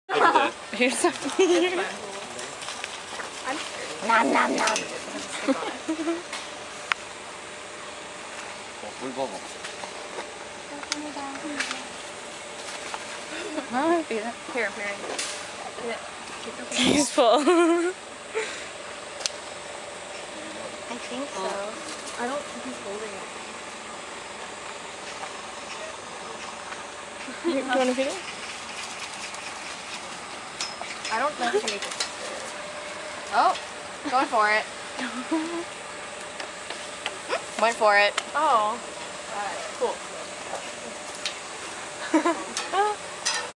Feeding Animals
Mary and Victoria feeding an animal at Seoul Zoo!